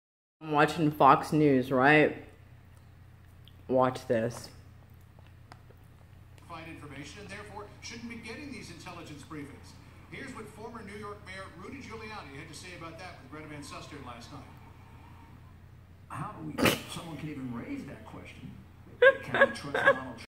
Man acts goofy behind Fox News reporter John Robert's on live TV!!!!
I was watching Fox News and this happened. A man walks behind  reporter John Roberts on live TV and plays with his nipples and flickers his tongue. Lol